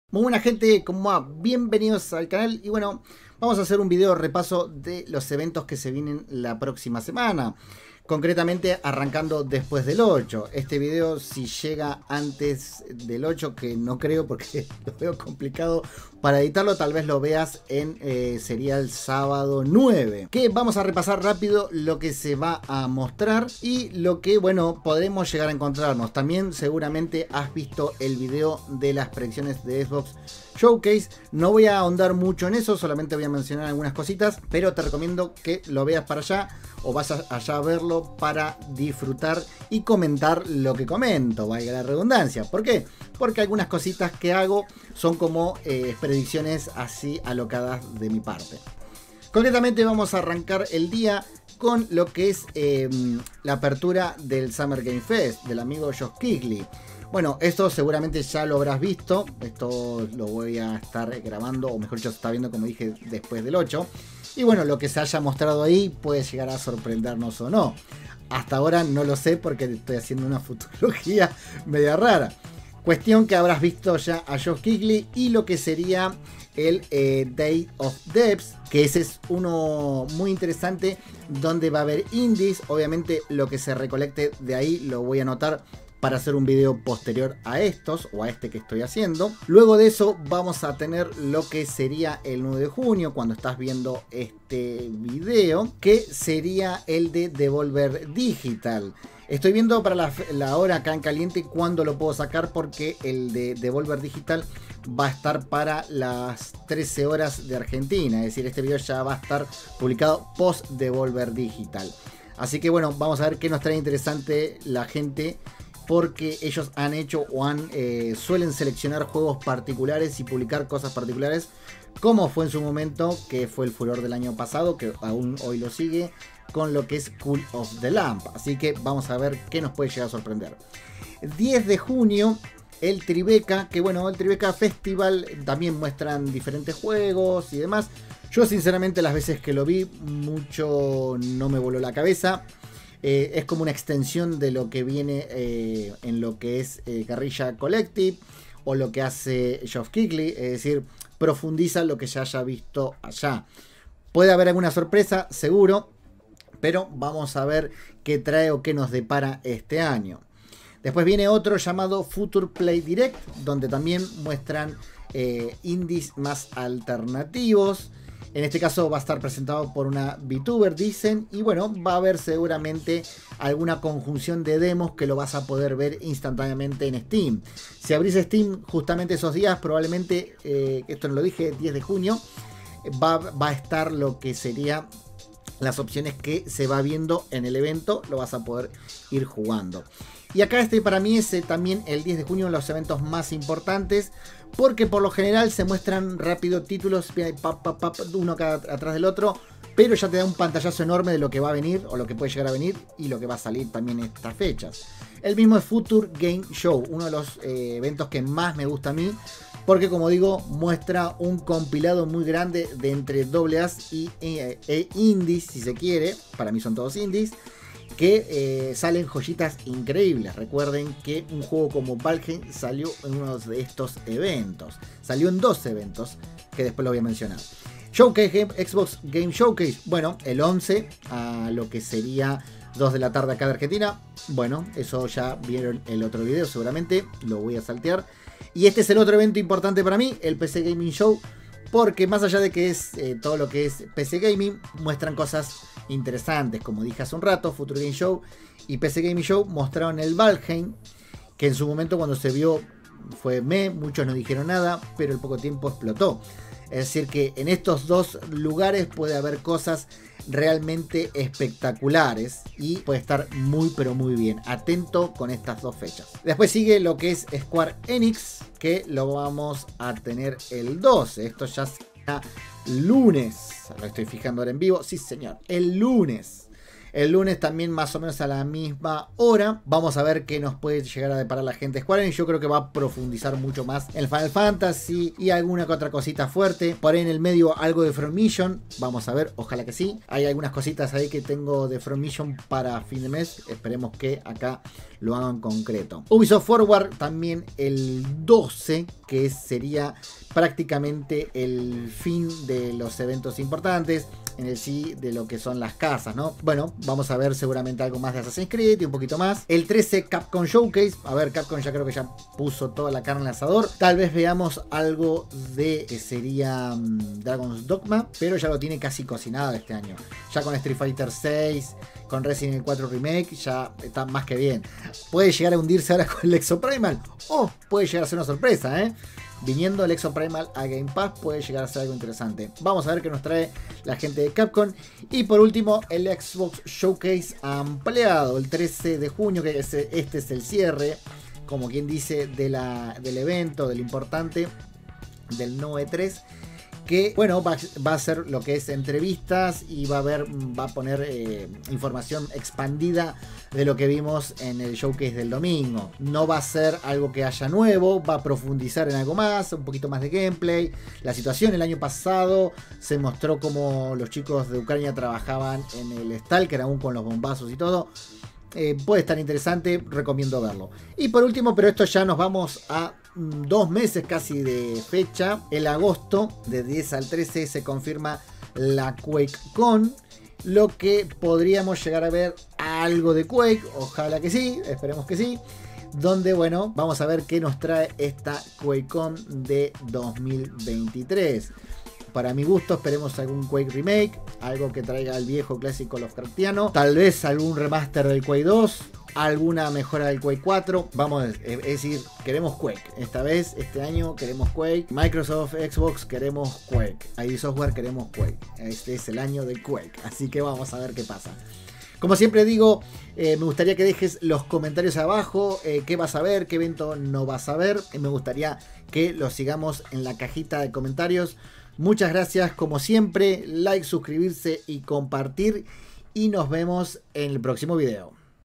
CALENDARIO SOBRE LOS EVENTOS DEL NO E3 | REPASO DE EVENTOS QUE SE VIENEN | PS5 - XBOX - PC
Les traigo este video con un repaso de los eventos que vamos a tener en estos dias, lamentablemente llega un poquitin tarde (apretada la agenda de cosas je) pero al menos espero que les guste este pequeño repaso de lo que viene una opinion agregada.

Se agradece tu like, compartir y comentar si te gusta este contenido

Codigo de Creador:
ZENKIUT